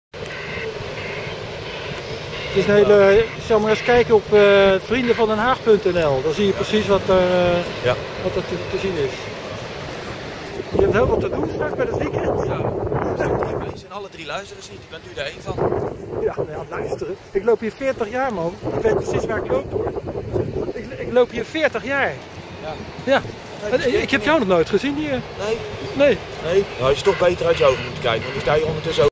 Luisteren? Ik loop hier 40 jaar man
Hoe mis kan het gaan met communicatie als je verboden erop zet terwijl ke GEVAAR bedoelt! hagazine.wordpress.com